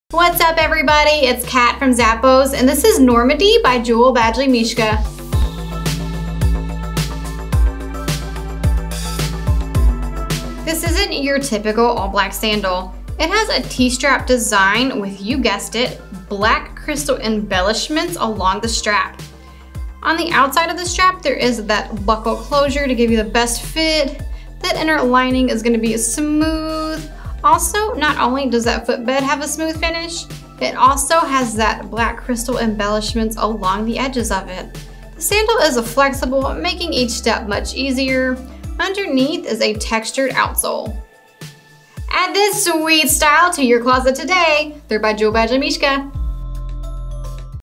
Jewel Badgley Mischka Normandy SKU: 9377719
The Jewel Badgley Mischka&#174; Normandy offers an eye-catching touch to a must-have style.  This breezy T-strap sandal is crafted with a man-made upper, and is completed with crystal embellishments for a sparkling finish.
Open toe.
Adjustable strap with buckle closure.
Lightly padded footbed.
Man-made sole.
Imported.